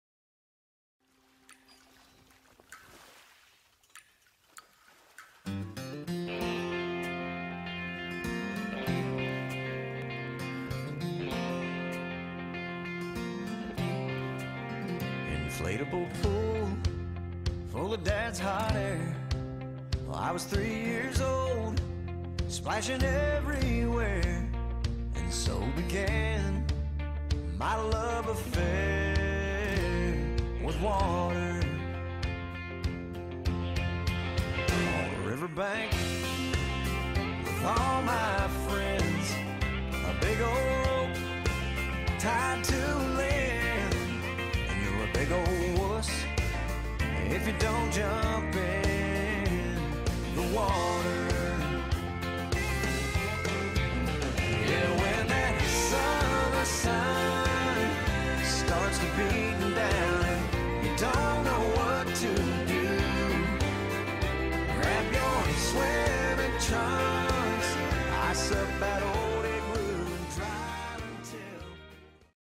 Sliding Rock, North Carolina
A quick edit of a recent trip to North Carolina. A very unique and exciting adventure!